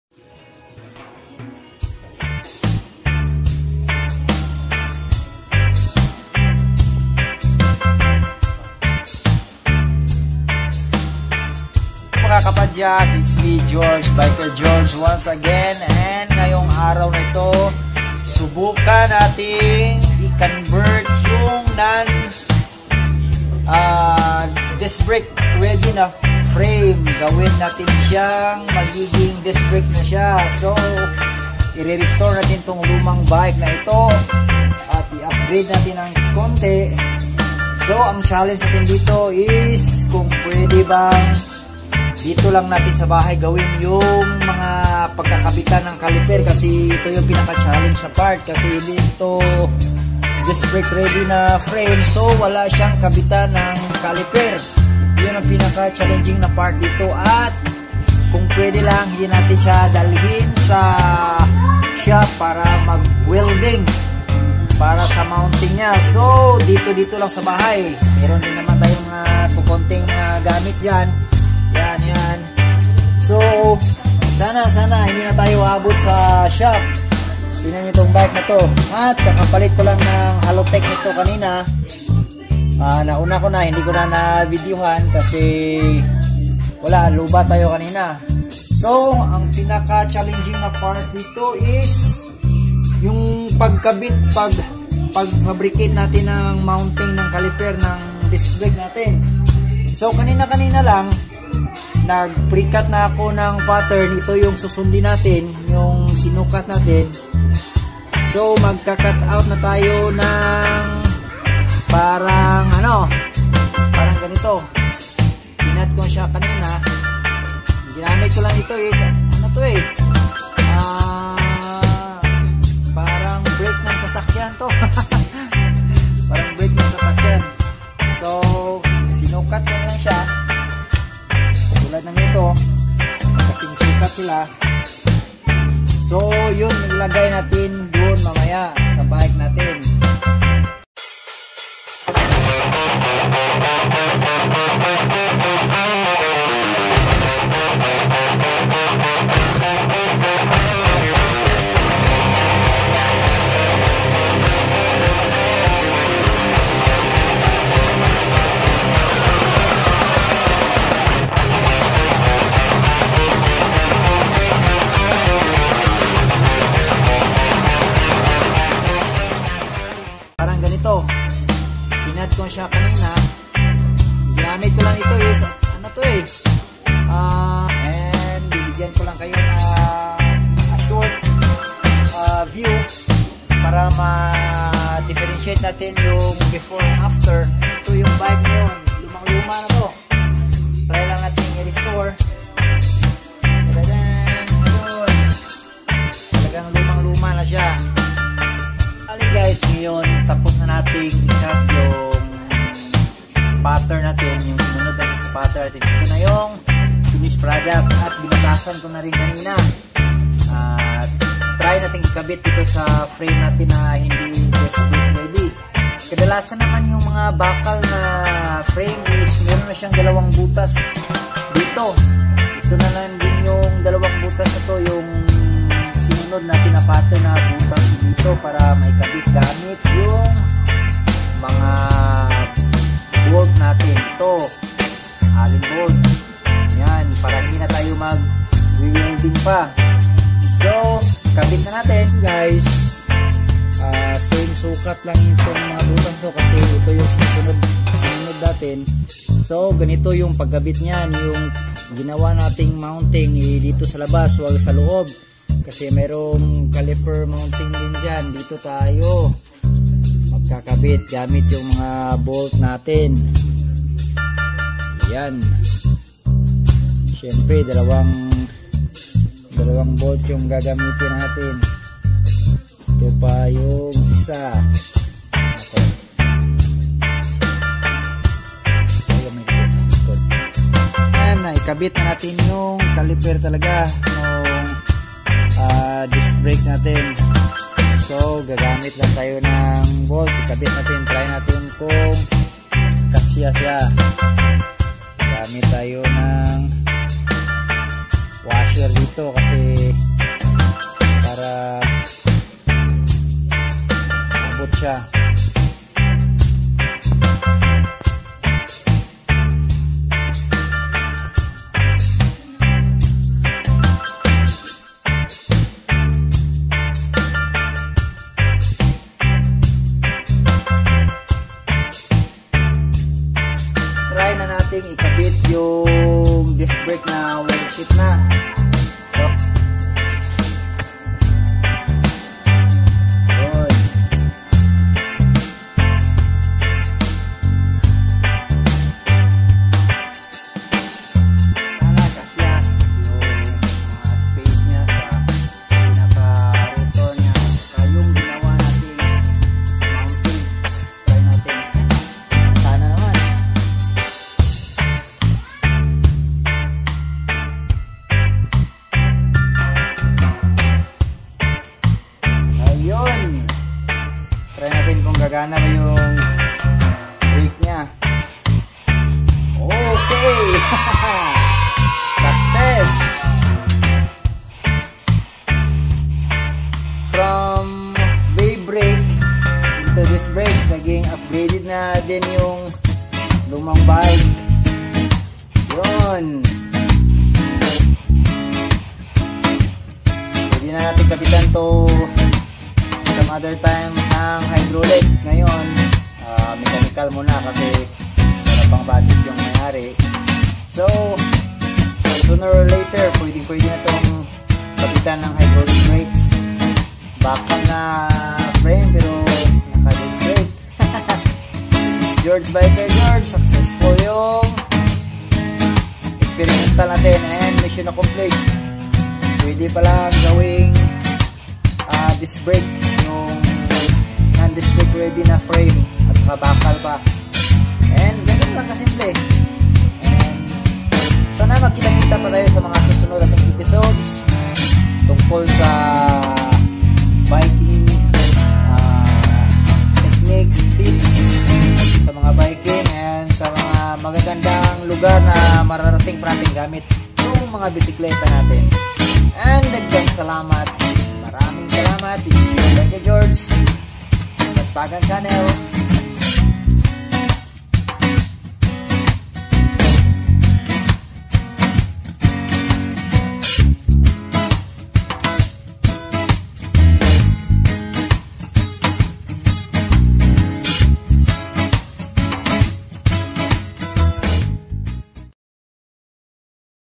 HOW TO CONVERT V BRAKE TO DISCBRAKE II RESTORING OLD BIKE II NON DISC BRAKE STEEL FRAME CONVERTED
Old school steel frame conversio
mission is to convert a non disc brake steel frame to a disc brake
without any shop touch
all the way D.I.Y
all the works is 100% inside the for
corner your house